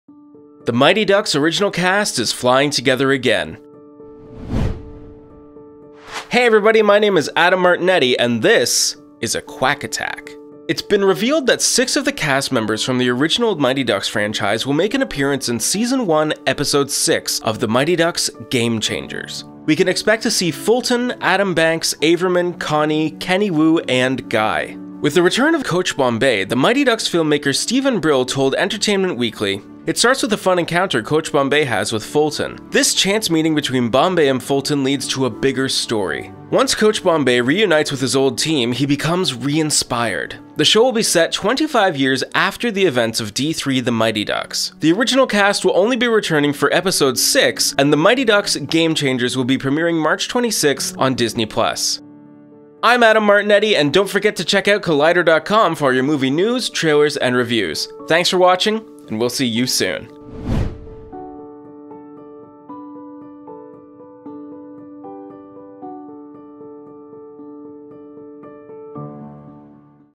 Mighty Ducks Original Cast Returning for an Episode of the Disney+ Reboot
The Mighty Ducks original cast is flying together again! In a report by Entertainment Weekly, it has been revealed that six of the original cast members from the original Mighty Ducks franchise will make an appearance in Season 1, Episode 6 of The Mighty Ducks: Game Changers. Elden Henson will reprise his role as Fulton, with Vinny La Russo as Adam Banks, Matt Doherty as Averman, Marguerite Moreau as Connie, Justin Wong as Kenny Wu and Garrett Henson as Guy.

With the return of Emilio Estevez in his role as Coach Bombay, The Mighty Ducks filmmaker Steven Brill tells Entertainment Weekly: "It starts with a fun encounter Coach Bombay [Emilio Estevez] has with Fulton — they have an interesting run-in on the street reminiscent of ways they used to meet each other back in the early days,” Brill continues that, ”This chance meeting between Bombay and Fulton leads to a bigger story."